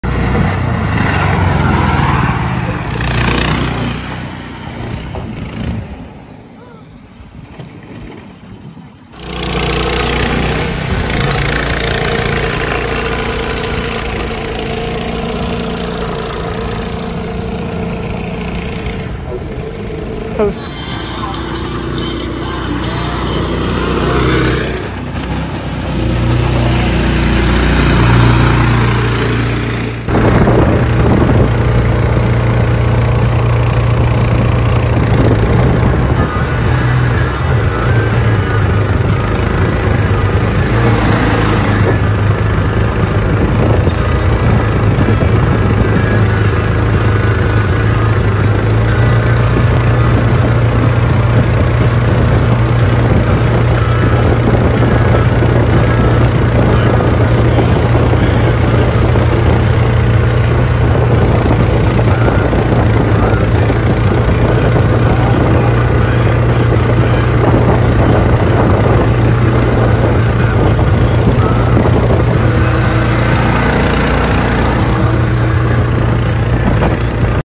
Mini surface drive pushes 1648 jon boat in FL 🌴
The Feather Lite surface drive kit fits a whole host of small engines, including our own 13.5hp and 15hp engine

Kit costs $1099 plus cost of electric start engine ($460-490)

Assemble yourself or have us build your drive and ship it ready to run